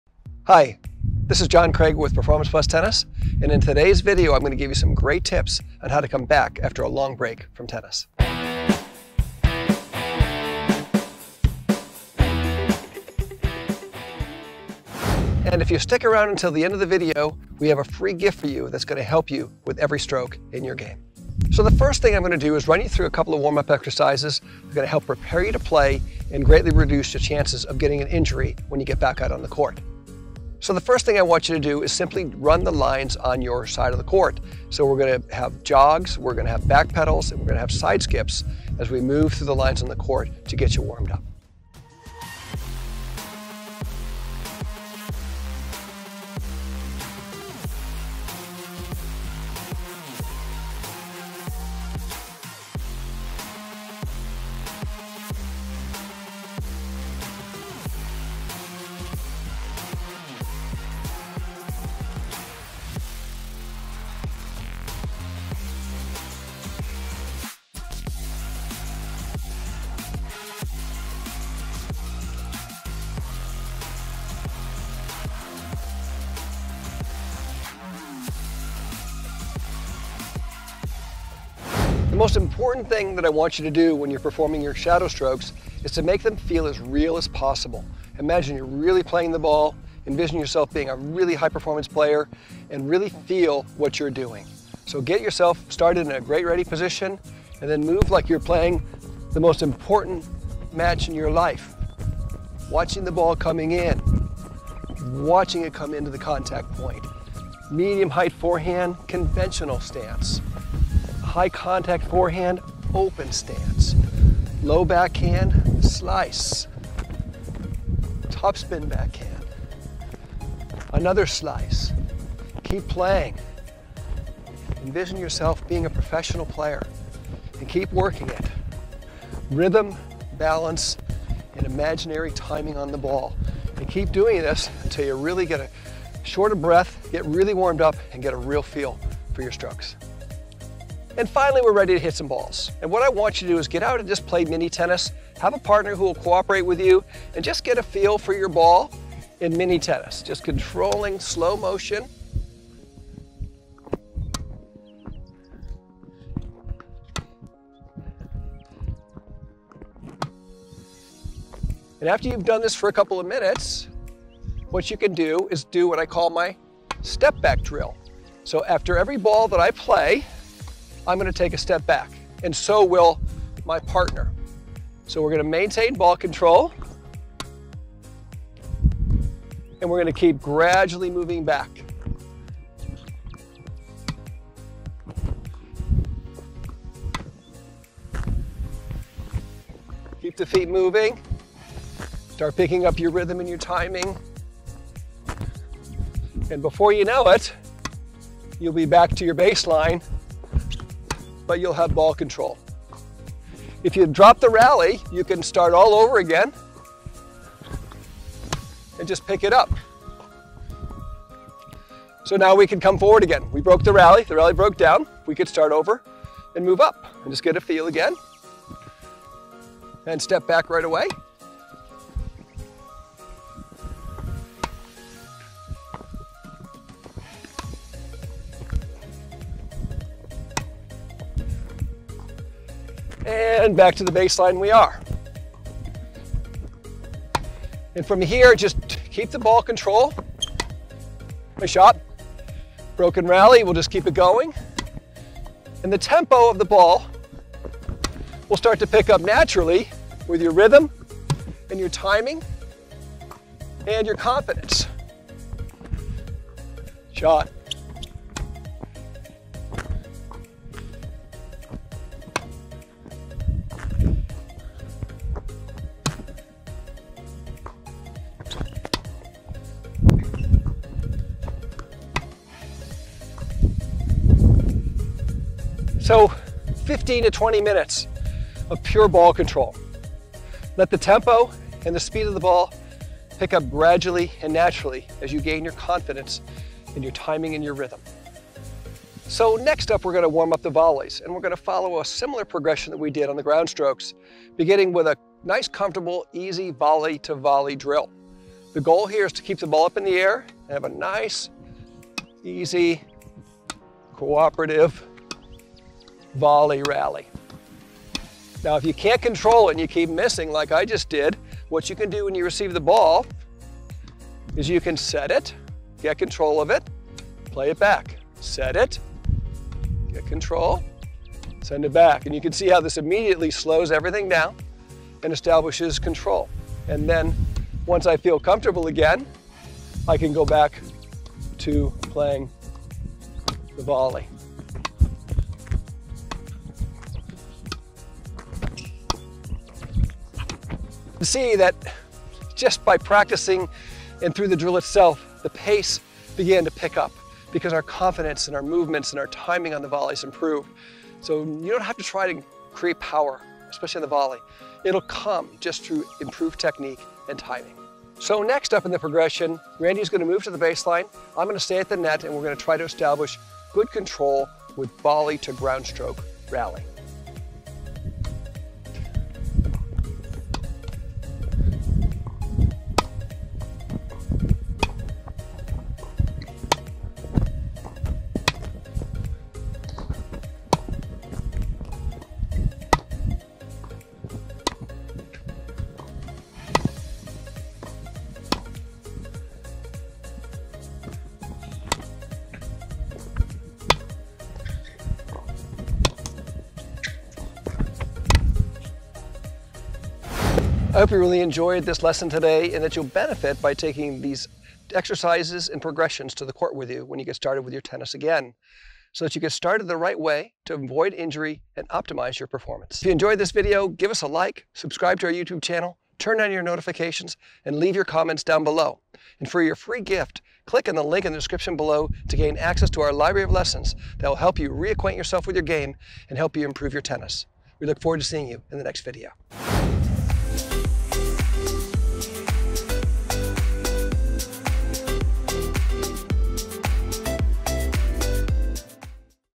Tips For Coming Back To Tennis After A Break
Before you play, make sure to ease into it with a nice warm-up. Run the lines on the tennis court and work on your movement going forward, backward, and sideways. Additionally, perform lunges and high knees. You can add any twist to this that you would like!
Shadow Swings- now that you are warmed up before you even hit a single ball, try shadow swinging your groundstrokes and work on moving side to side and hitting different contact points.
Ball Control- Grab a partner to play with and work on gradually moving backward from the service line to the baseline. Move back slowly, and if you miss, reset and start at the service line again.
Volley to Volley- With your partner, work on hitting volleys back and forth and also work on setting the ball up in the air before hitting your volley.
Volley to Groundstroke- Lastly, take turns with your hitting partner and work on the volley to groundstroke. After you have volleyed, hit groundstrokes, and vice versa.

The most important thing to remember is to take it slow. Ease back into tennis and have fun!